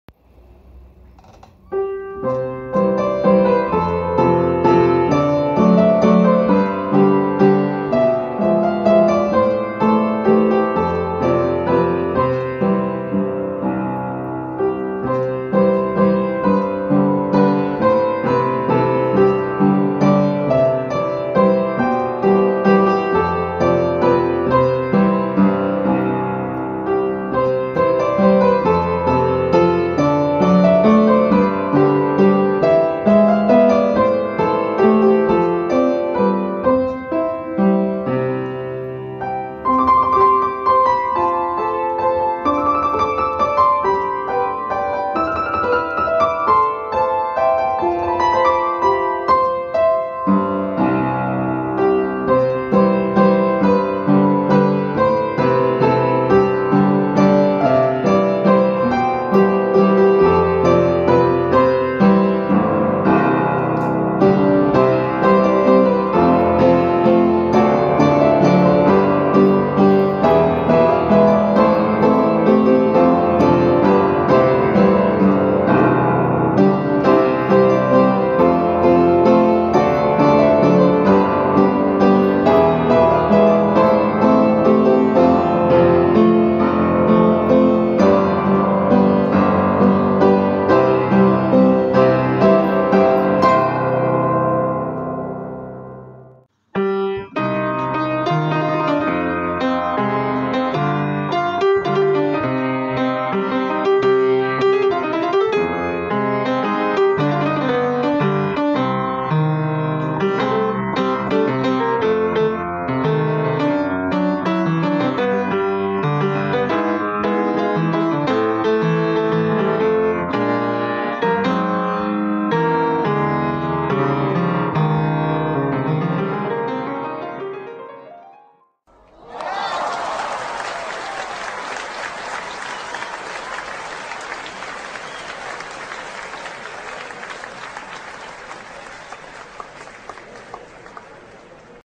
Christmas & Happy New Year greeting from Delysia piano sheet user & Christheo Music Arranger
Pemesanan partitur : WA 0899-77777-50
Seorang user partitur dari Bekasi memberi ucapan selamat Natal & Th Baru lewat permainan solo piano yg direspon oleh Christheo Music dg aransemen yg sedikit berbeda. Ini adalah kolaborasi kedua antara user partitur Christheo Music sbg provider partitur musik/music sheet. Delysia adalah murid Jaya Suprana yg mendalami Russian Classical Music yg terkenal cukup rumit dari sisi teknik.